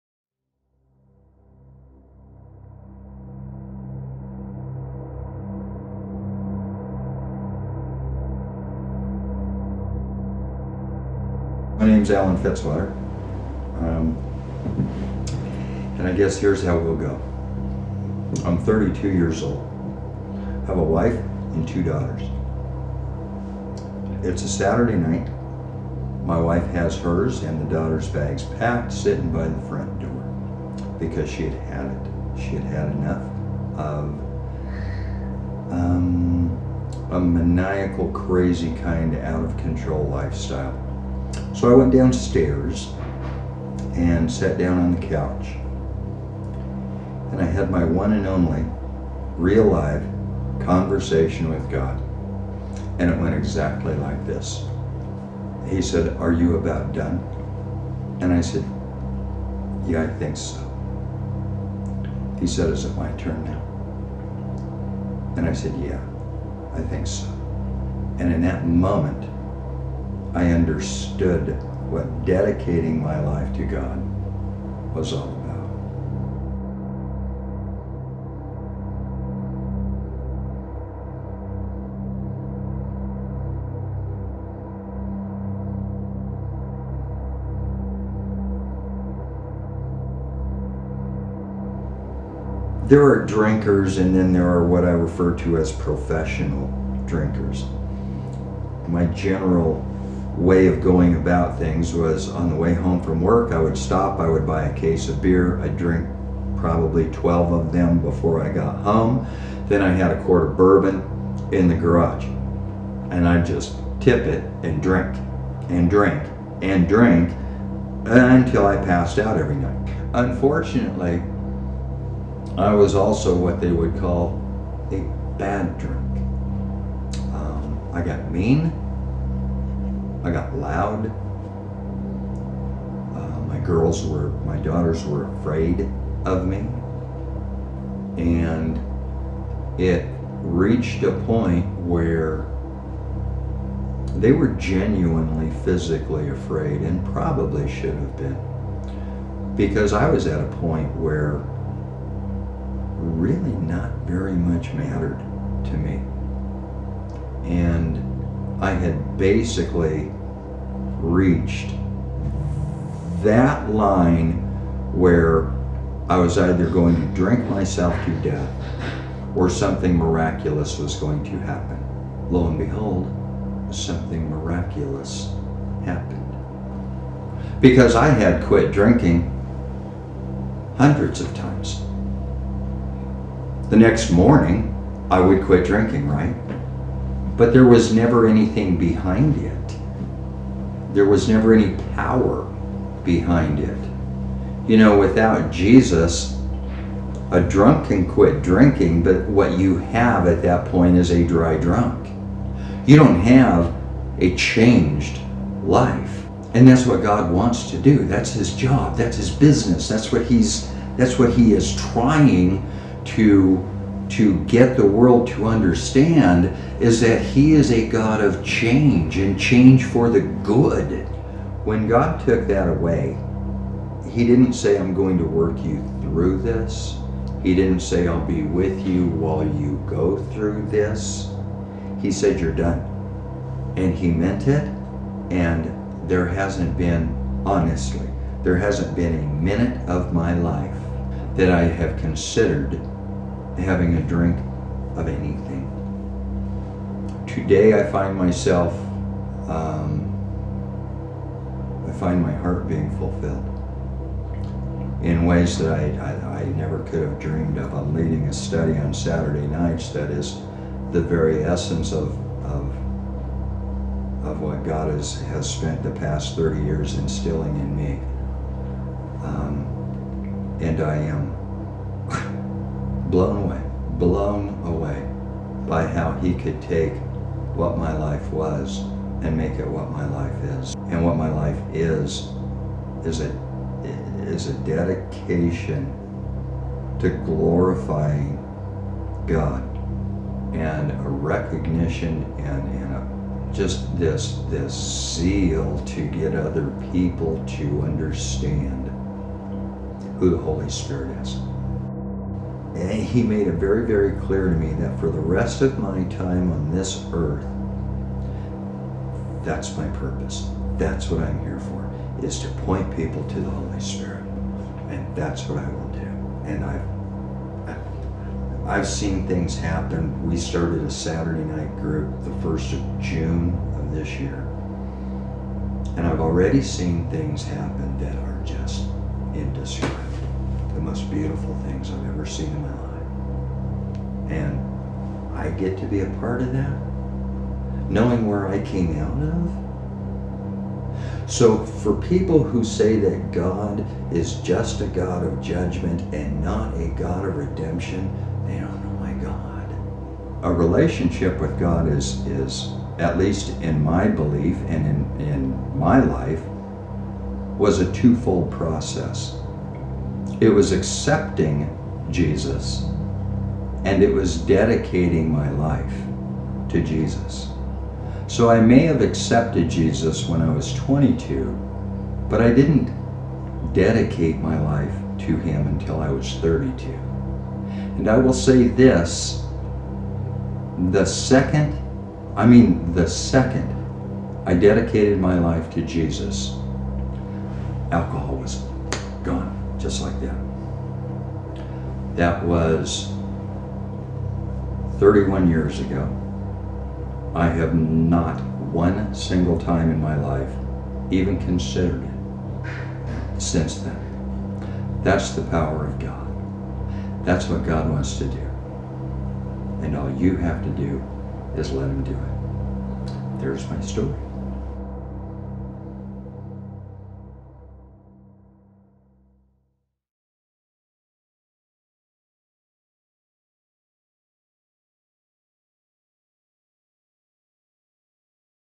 Alan Fitzwater // A Tale of Redemption
At 32 years old, his wife had her daughters and bags packed, ready to leave. In an unforgettable "wake up" moment, God called Alan to leave his past of alcoholism behind to follow the Lord.

Listen to Alan's story and be encouraged by the massive work of freedom God can move in a person's life, even under the greatest chains of addiction. God gives us the power to overcome!!!